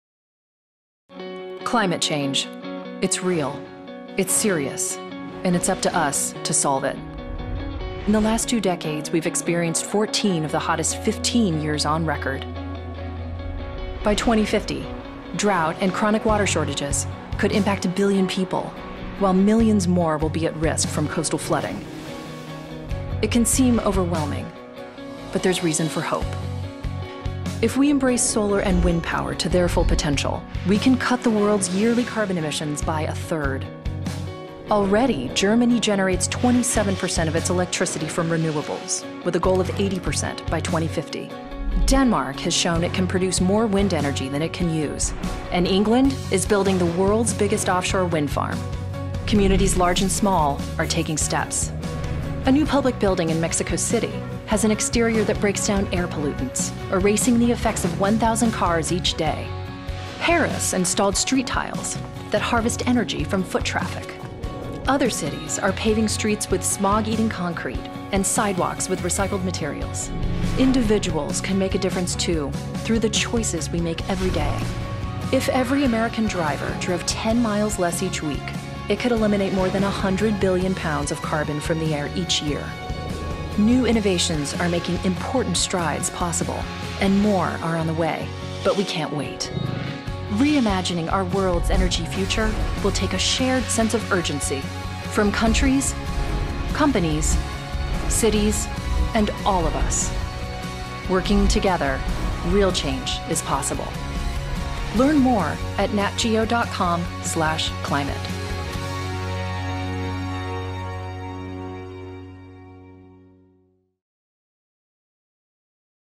Climate Change: It’s Real. It’s Serious. And it’s up to us to Solve it. | National Geographic
National Geographic is partnering with the United Nations Foundation and the Earth To Paris coalition to give a powerful voice to a critical message: Reimagining our world’s energy future will take a shared sense of urgency—from countries, companies, cities, and all of us.